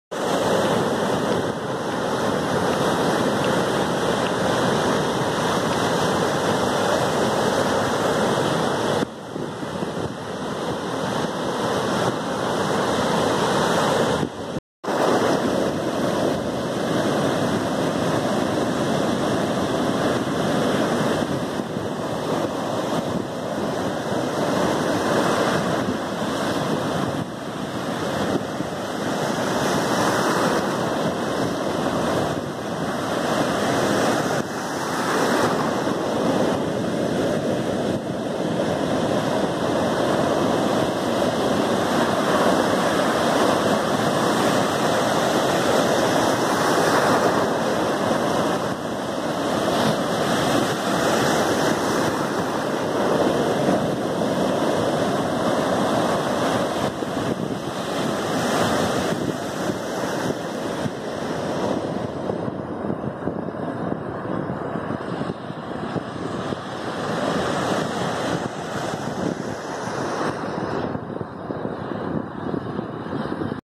The waves at Falasarna Beach
Filming the waves at the famous Falasarna Beach, in Crete.
Shot on 4K on a Moto G5S Plus smartphone, in two different segments on the same day [21/6/2020]